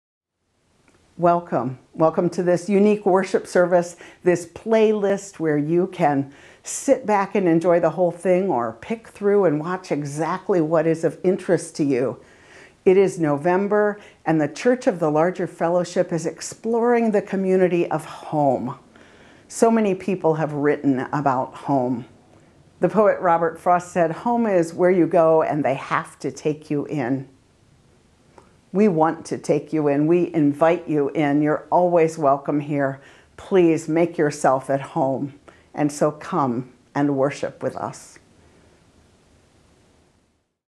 November Welcome - Rev. Meg Riley
Rev. Meg welcomes you to worship.